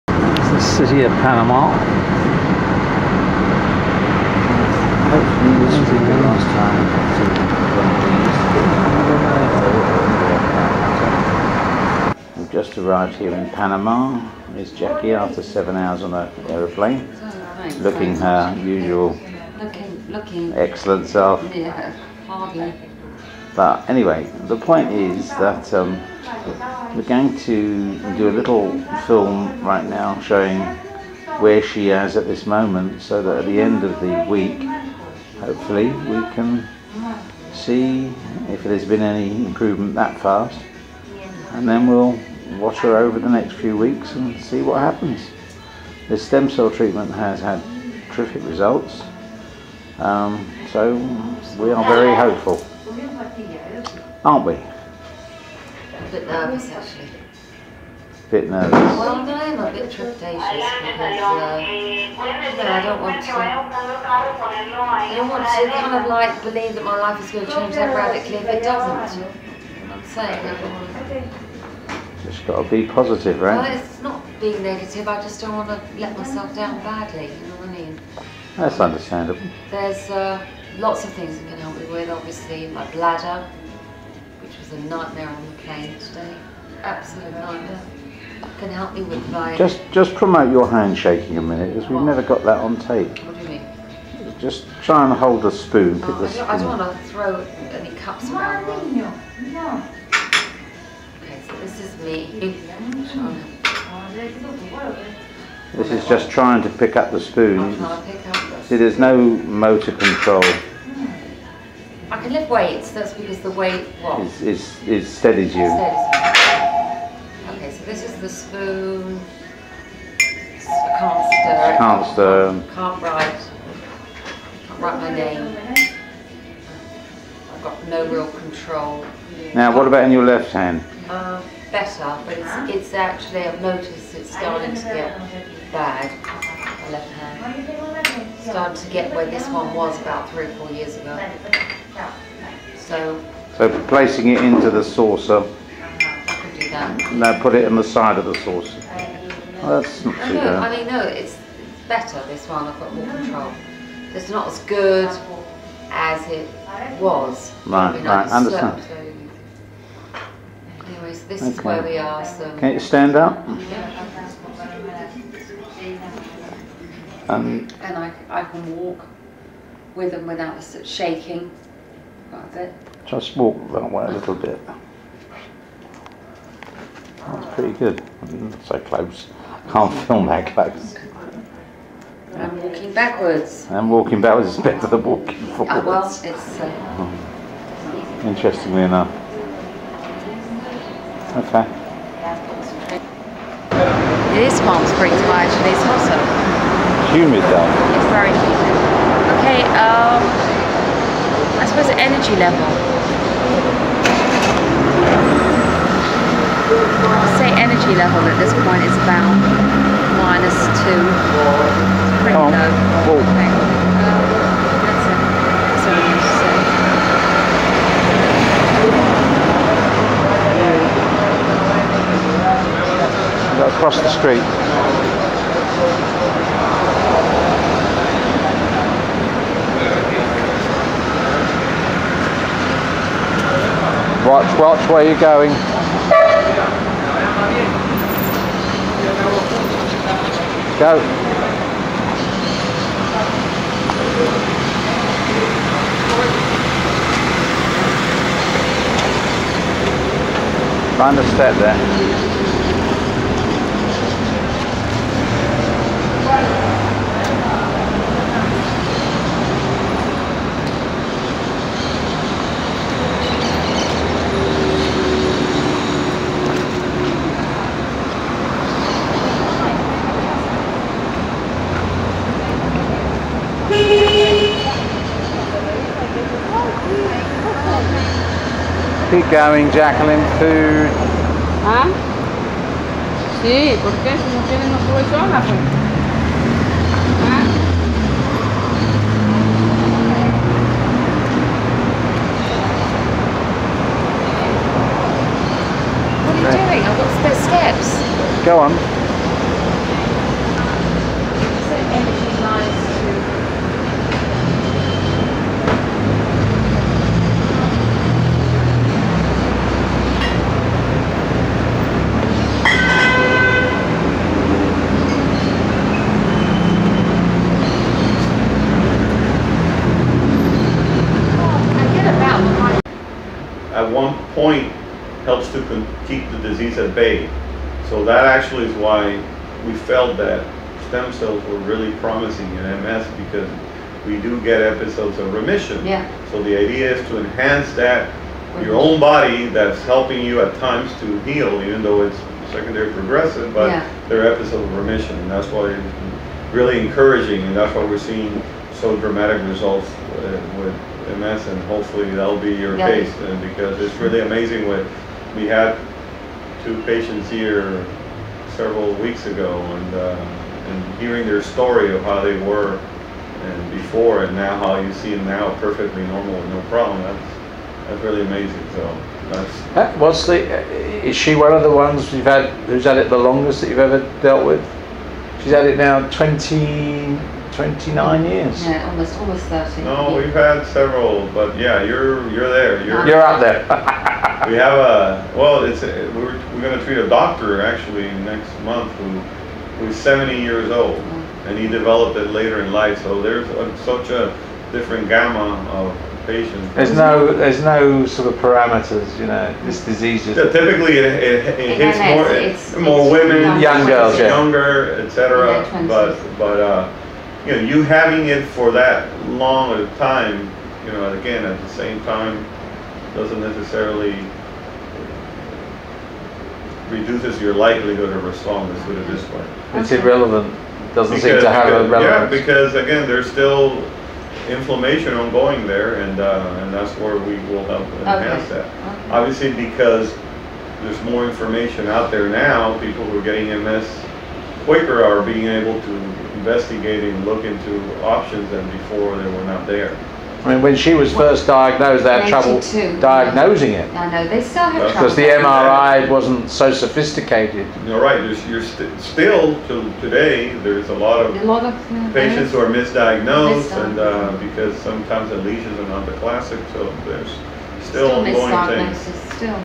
stemcell Therapy, Day 1, Panama City, Panama.
Jacalyn enters a Stem Cell program available only in Panama, Costa Rica and China, due to FDA time constraints on approval,  developed by Scientists and Doctors in San Diego CA!

The program uses your own Stem Cells extracted from body fat and Umbilical Stem cells obtained from donors (non feotal).